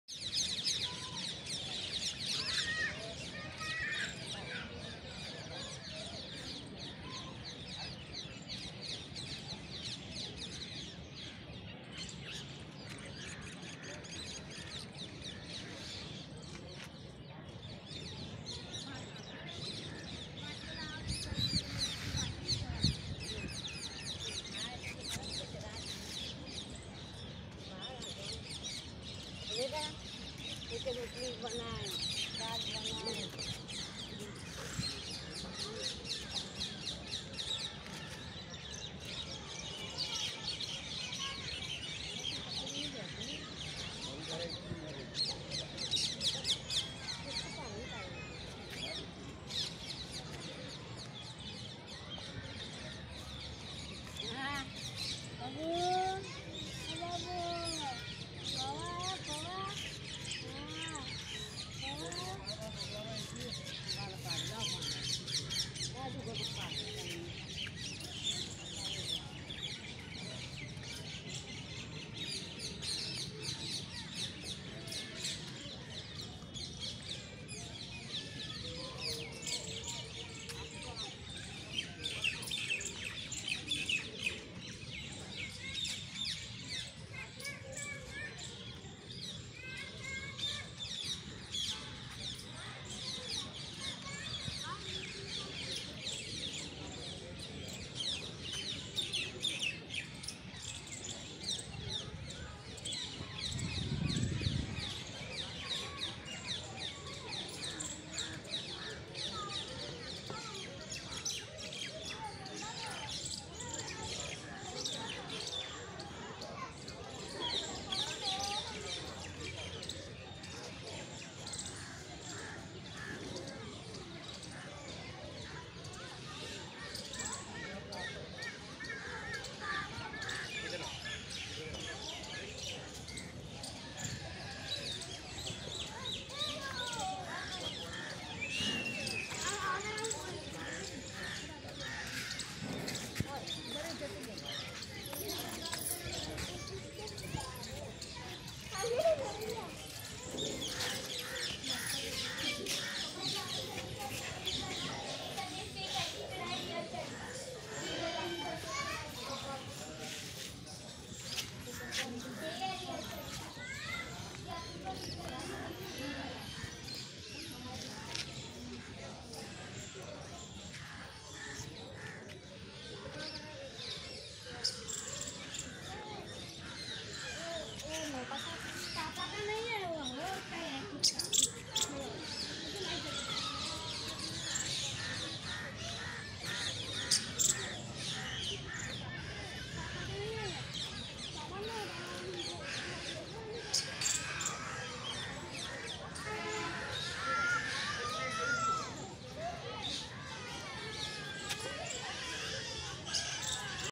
Sabarmati Ashram - Ahmedabad, Gujarat.
The Sabarmati Ashram is situated at the Bank of river Sabarmati, Ahmedabad.
This ashram was home to Mahatma Gandhi from 1917 to 1930 and served as one of the main centres of Indian freedom struggle.